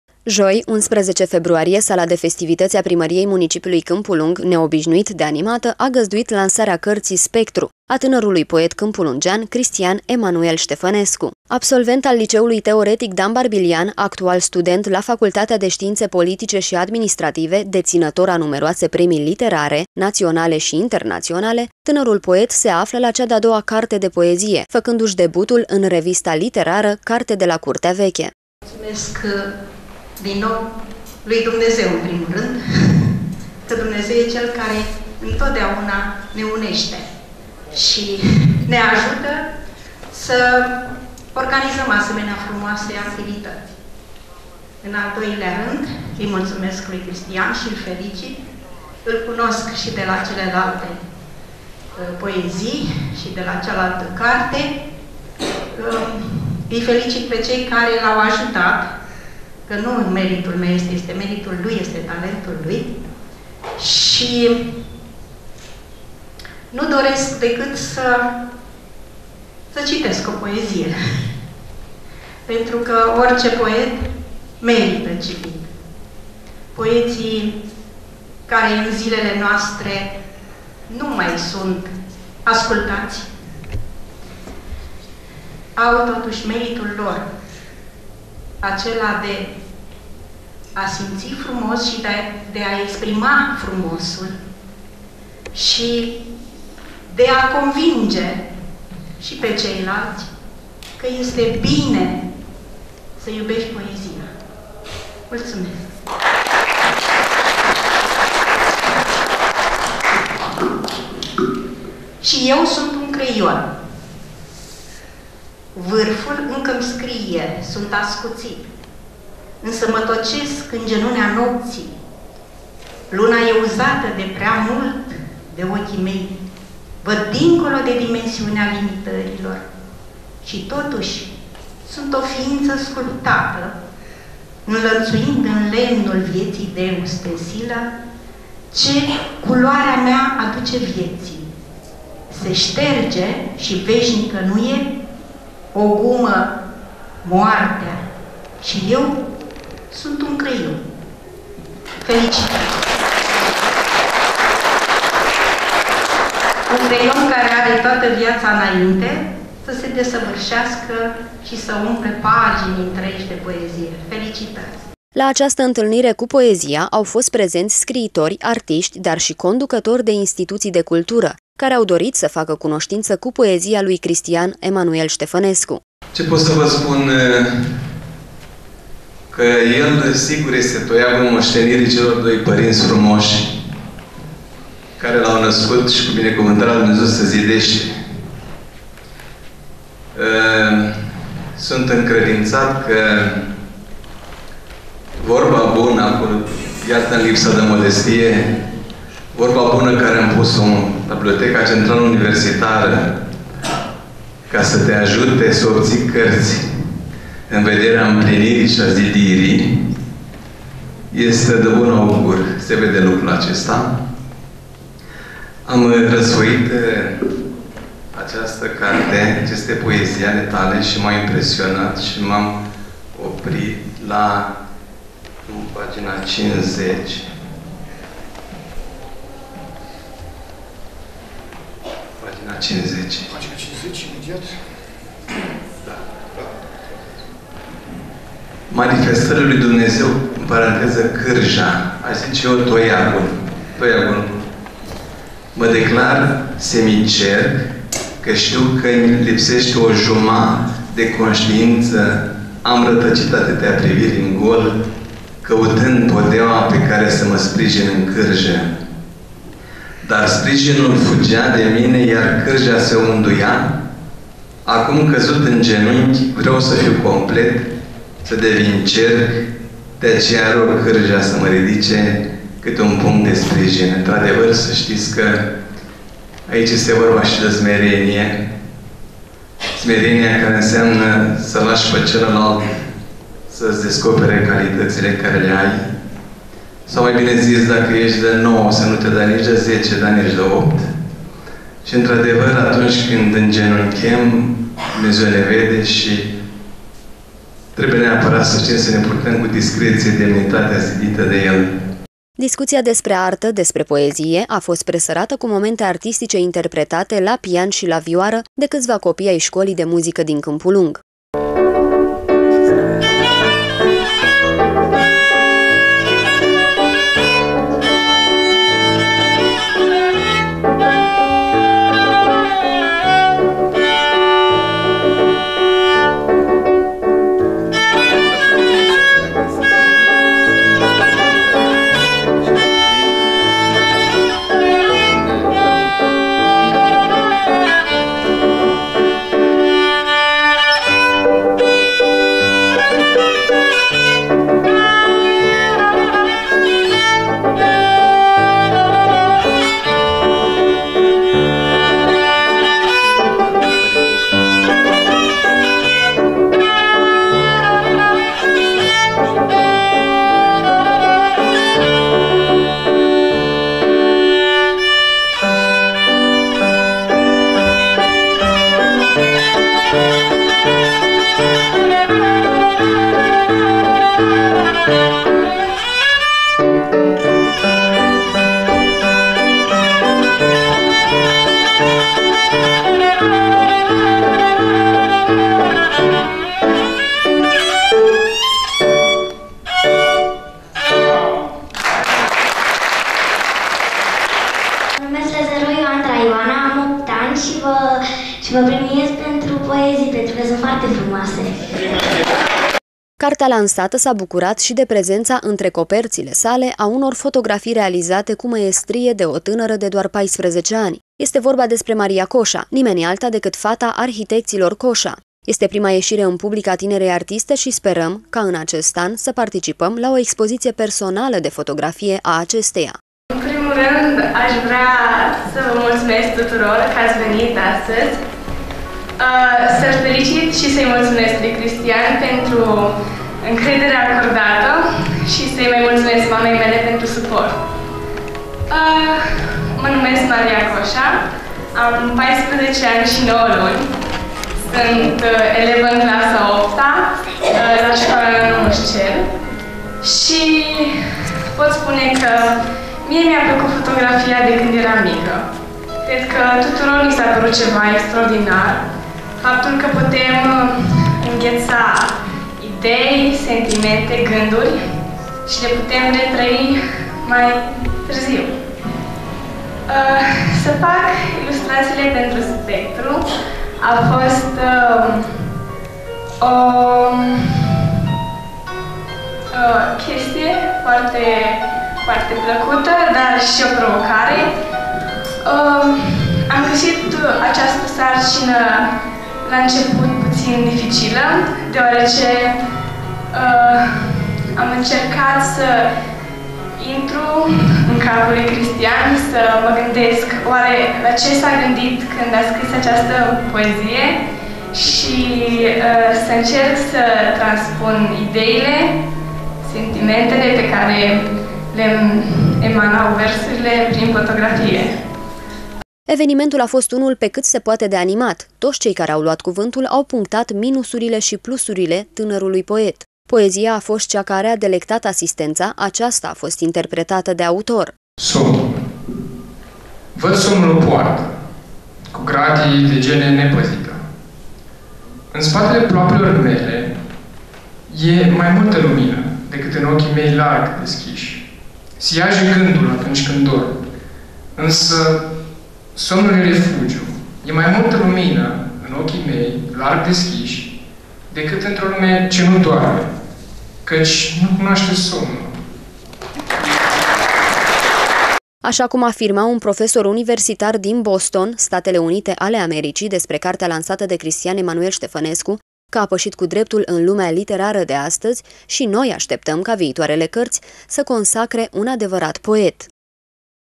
stire 12 02 2016 lansare carte cristian stefanescu
Pentru iubitorii de poezie, lansarea cărţii „Spectru”, a tânărului poet Cristian Emanuel Ştefănescu, a fost un prilej de a pătrunde în atmosfera acestei arte. Sala de Festivităţi a Primăriei, neobişnuit de plină, la un astfel de eveniment, a găzduit artişti, scriitori, oameni de cultură, dar şi prieteni ai autorului. Momentul a fost presărat cu melodii interpretate la pian şi la vioară, de elevi ai Şcolii de Muzică din Municipiu.